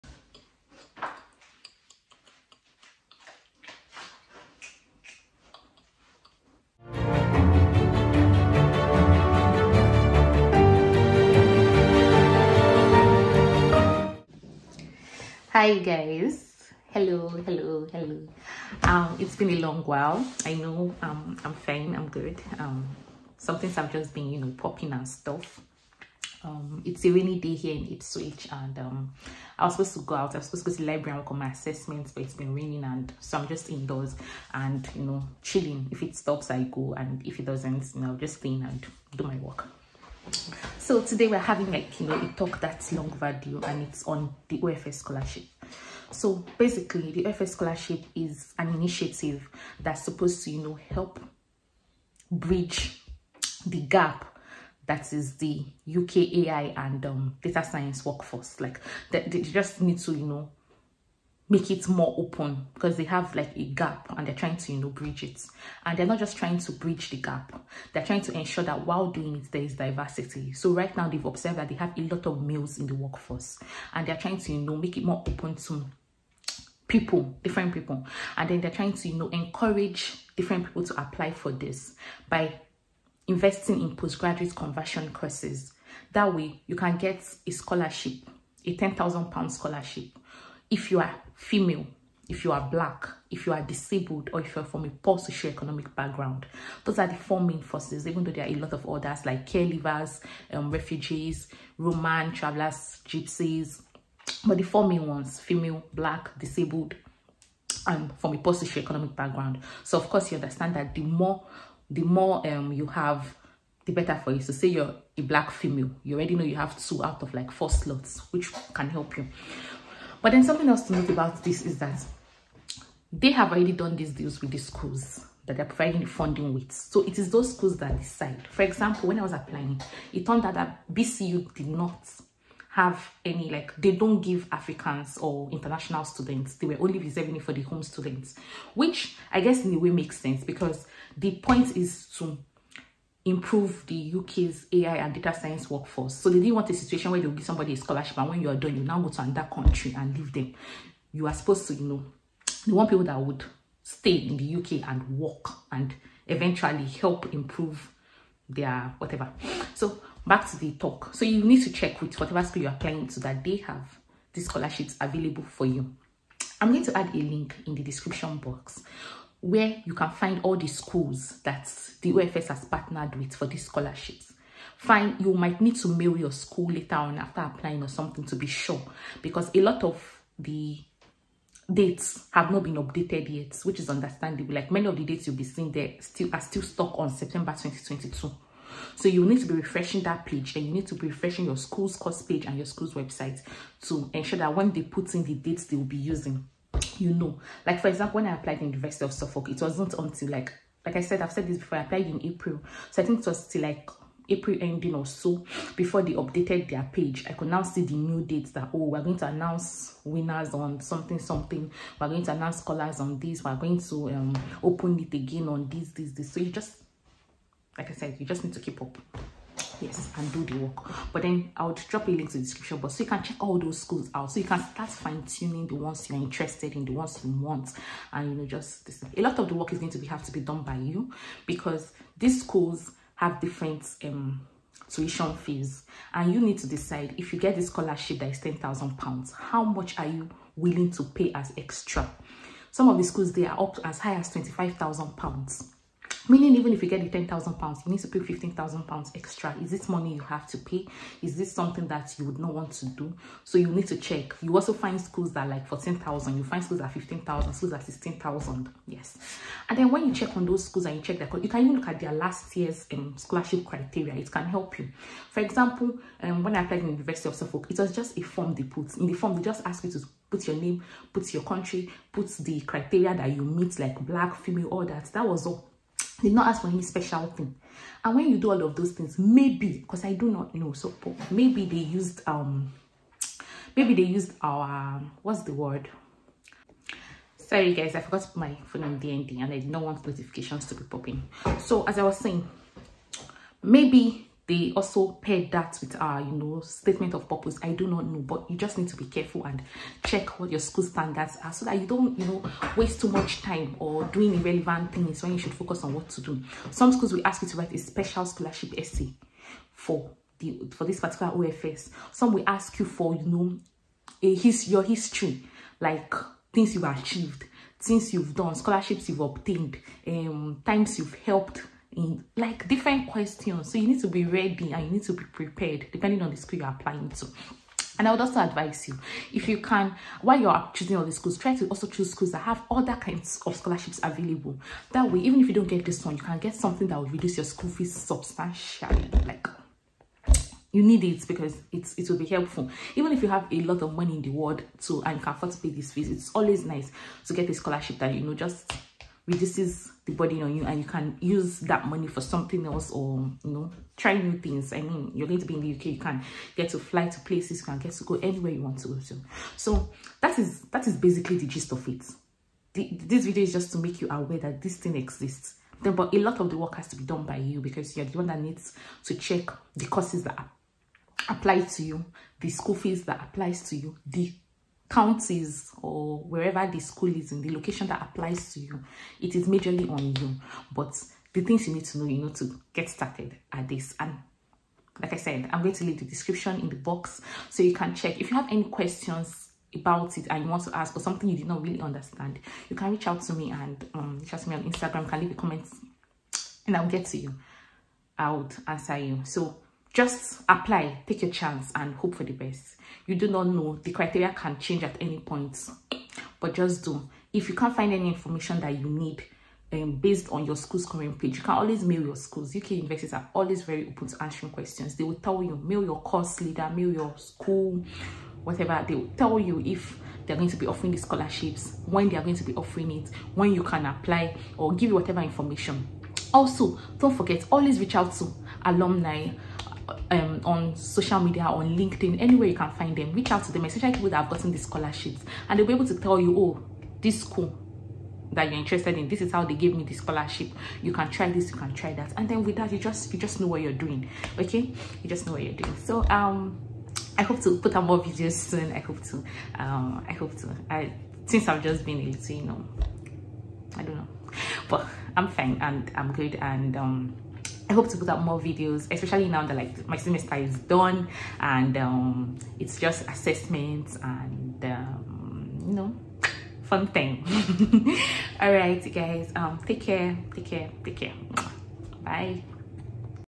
I GOT THE OFS SCHOLARSHIP FOR DS AND AI AT THE UNIVERSITY OF SUFFOLK| Learn all about it here!!!
Hi People,

Welcome to my YT channel! Thanks for being so patient while I was away.

I’ve finally made and put up this long-awaited video on the OfS Scholarship for Data Science and Artificial Intelligence courses.

Best of luck, Dearies😊

Muah 💋